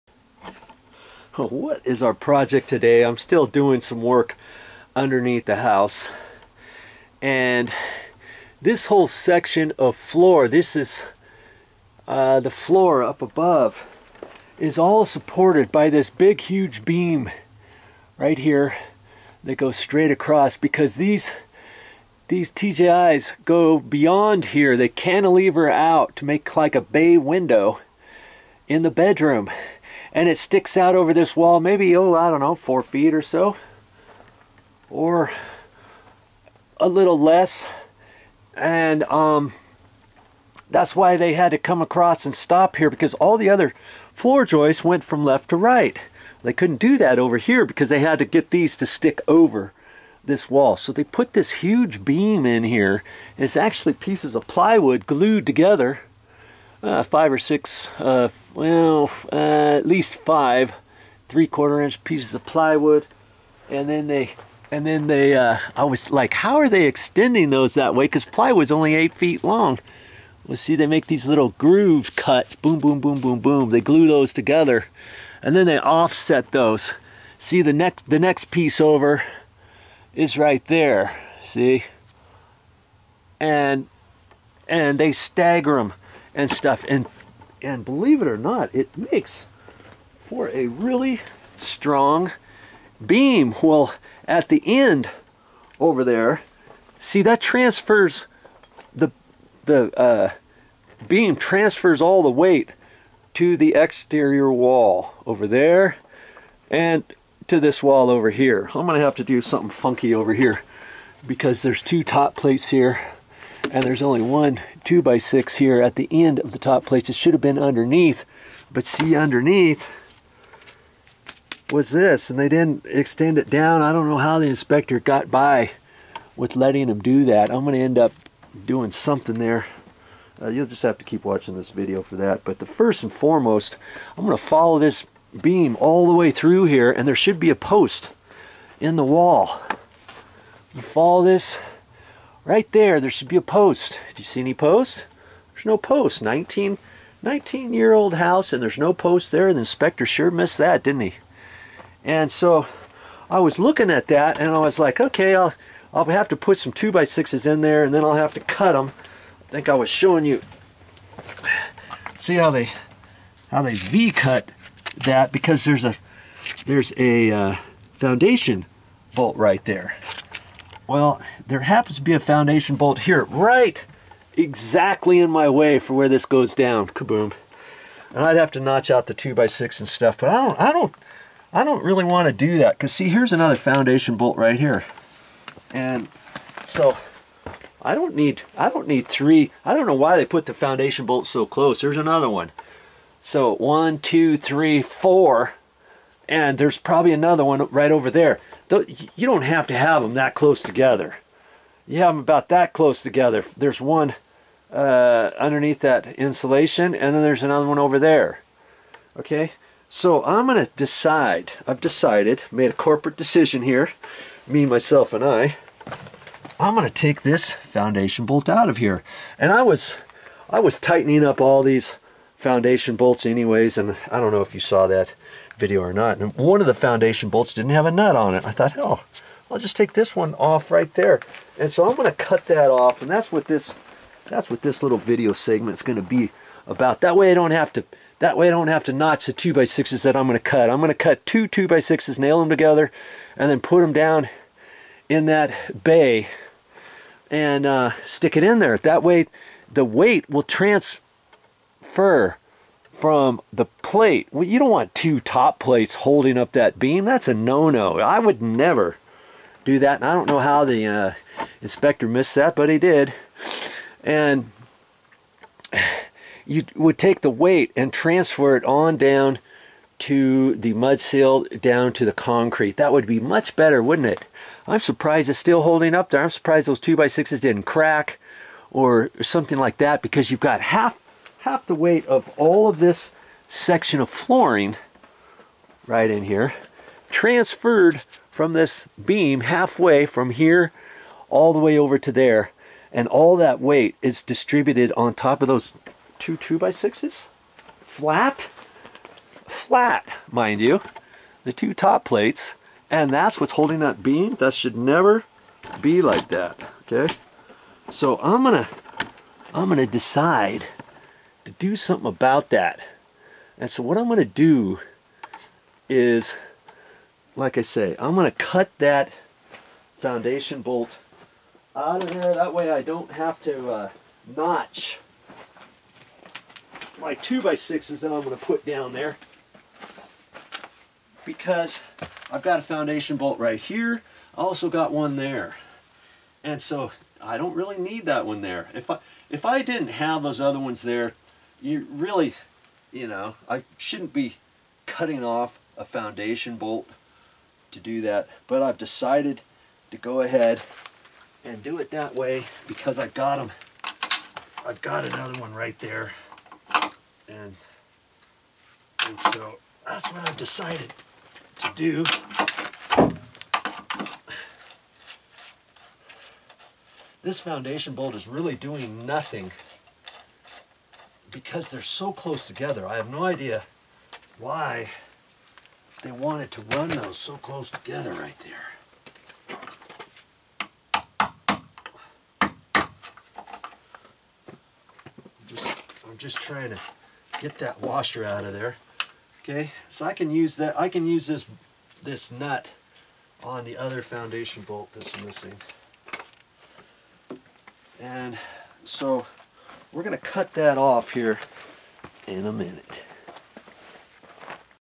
Shouldn't ALL Load Bearing Beams Have Support Posts Under Them? So Why Doesn't THIS One?!!...Part 1
Shouldn't all load bearing beams have support posts under them? Yes!...so why doesn't this one?...Part 1...good question!...is what this DIY howto video is about. VideoJoe is doing some inspecting under his house & he just noticed that there is a load bearing beam that doesn't have any posts under it to hold the weight of the beam which then holds up the weight of a section of the floor above! This house was built over 20 years & he's shocked that the Building Inspector never caught it on his framing inspection sign off! I'm now wondering what VideoJoe is going to do about it. The floor appears to be holding up fine but still. Watch & learn as VideoJoeShows you what VideoJoeKnows...should all load bearing beams have supporting posts under them?...of course!...so why doesn't this one?...Part 1... Ah...!